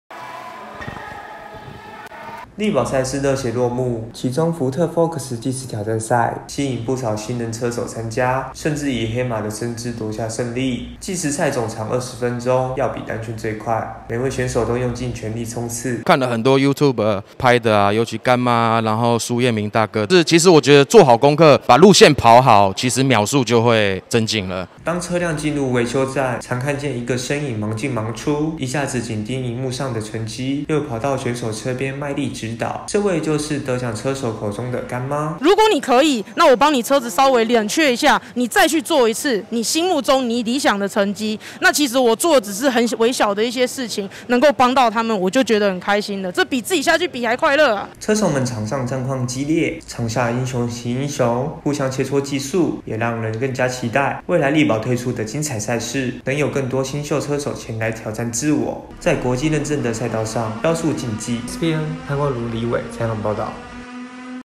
Focus計時賽戰況激 新人車手黑馬之姿奪冠
麗寶賽事日前熱血落幕，其中Ford Focus 計時挑戰賽，吸引有不少新人車手參加，甚至以黑馬的身姿奪下勝利，時賽總長20分鐘 要比單圈速度最快，拚車速同時考驗車技。

00:00 Focus計時賽戰況激 新人車手黑馬之姿奪冠
00:24 拚速度秀車技 駕駛指導有眉角
00:47 盼未來賽室 新秀車手再圓國際賽道夢